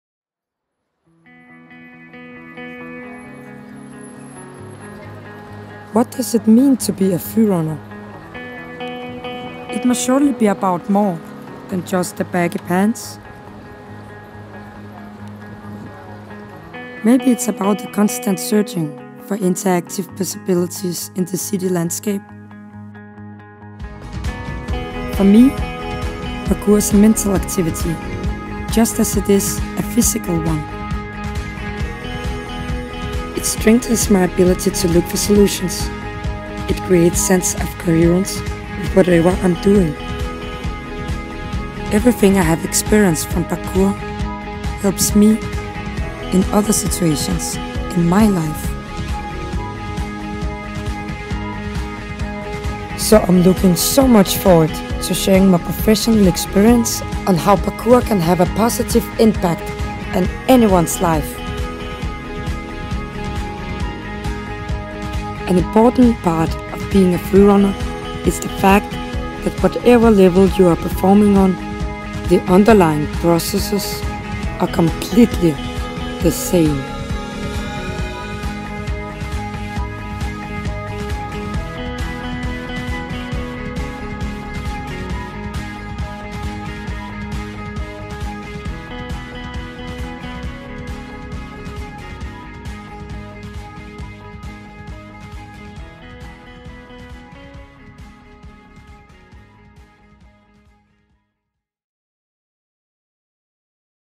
Natalia Ivanova FAMJAM Teaser 2014
This year, the founder of StreetExpression Natalia Ivanova, will attend FAMJAM i München, with an interesting presentation.

Don miss it.